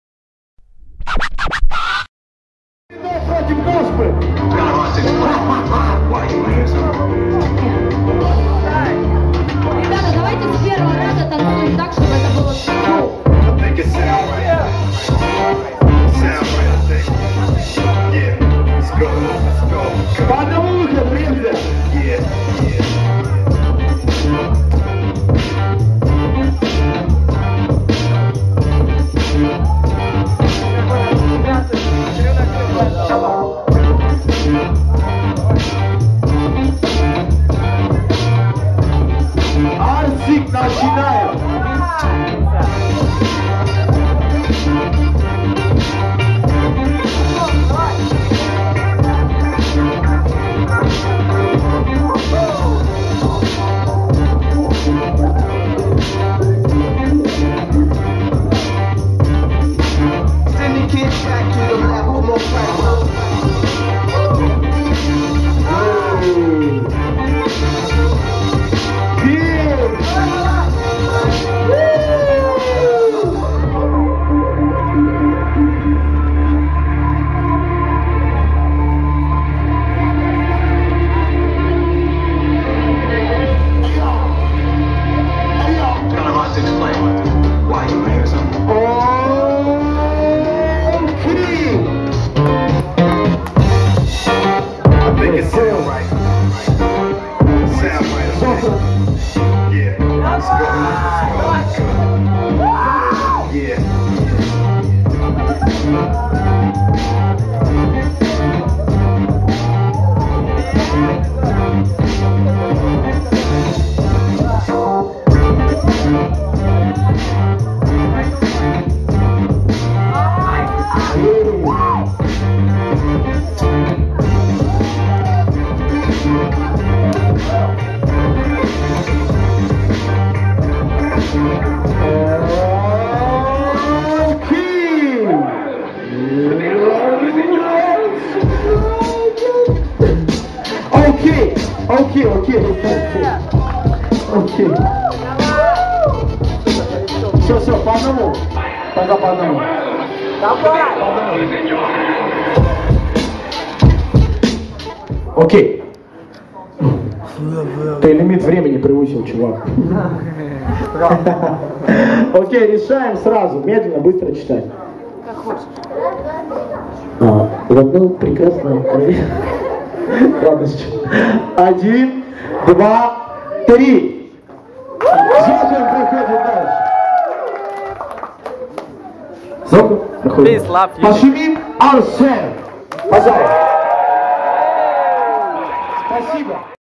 Arsen vs Zohan - Place and Action - Hip Hop dance 1x1 pro
29 марта 2014г.
г. Волгодонск